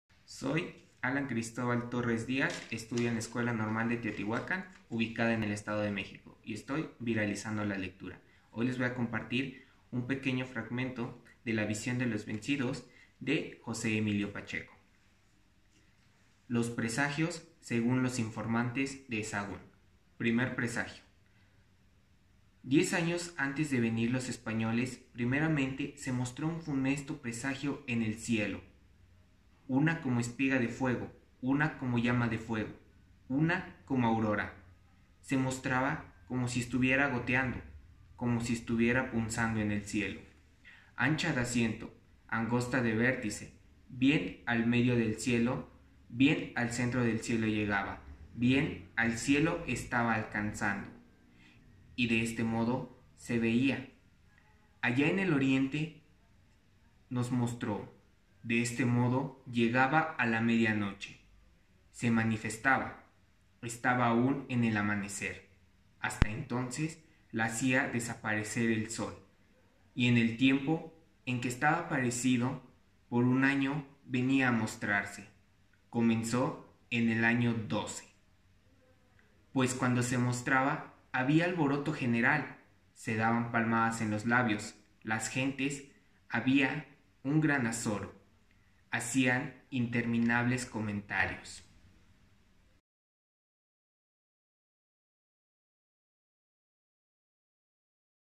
ViralicemoslaLectura Normal Teotihuacán EdoMéx
Título del Libro: La visión de los vencidos
Autor: Jose Emilio Pacheco
Nombre: Alan Cristóbal Torres Díaz
Institución: Escuela Normal de Teotihuacán
Estado: México